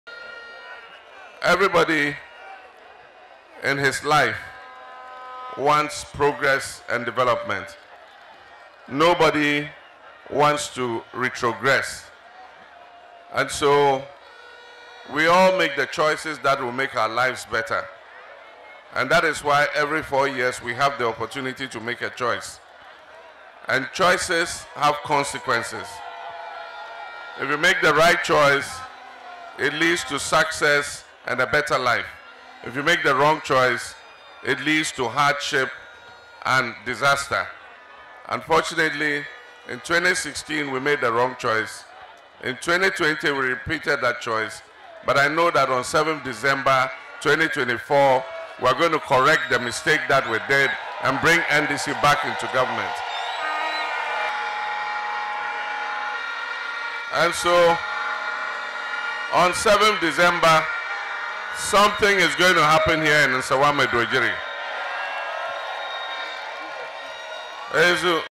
Correct Mistakes Made In 2016 and 2020 Elections - Mahama
ElectionHub: Correct the past mistakes of wrong choices made in the 2016 and 2020 elections... - Mahama urges voters